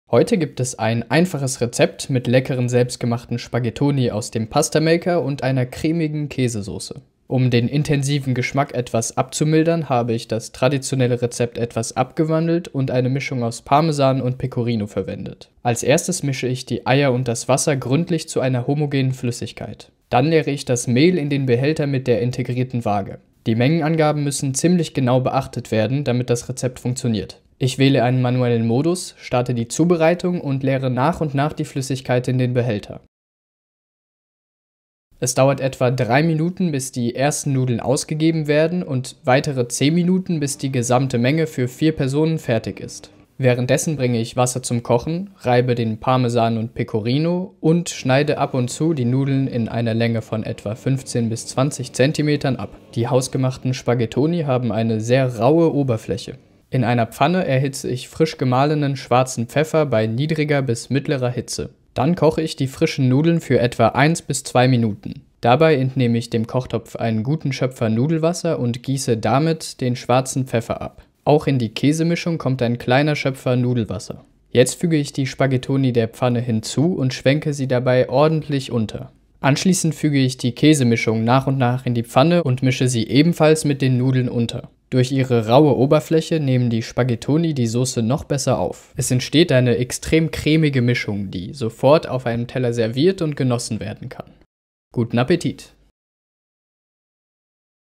Pastamaker Rezept: Spaghettoni in extrem cremiger Käse-Pfeffersauce
In diesem Video koche ich klassisches Rezept aus dem alten Rom mit einer cremigen Käsesoße (Cacio e Pepe). Die Spaghettoni (Dicke Spaghetti) hab ich mit dem Pastamaker Pastarella von Rommelsbacher hergestellt.

ZUTATEN (4 Personen)

Für die Spaghettoni mit dem Pastamaker:
400 g Mehl Typ 405
150 ml Flüssigkeit (2 Eier + Wasser)

Für Die Käsesoße:
Frisch gemahlener Schwarzer Pfeffer
150g Pecorino Romano
150g Grana Padano (oder Parmigianno)

ANLEITUNGEN

Die Spaghettoni mit dem Pastamaker herstellen und 1 - 2 Minuten kochen.

Den Pfeffer bei niedriger bis mittlerer Hitze in einer Pfanne erwärmen, mit Nudelwasser ablöschen und die Spaghettoni dazugeben und unterschwenken.

Den Käse Reiben, mit etwas Kochwasser vermischen und in die Pfanne mit den Nudeln geben. 2 - 3 Minuten gründlich rühren, bis die Soße eine cremige Konsistenz hat und überall verteilt ist.

Servieren und genießen!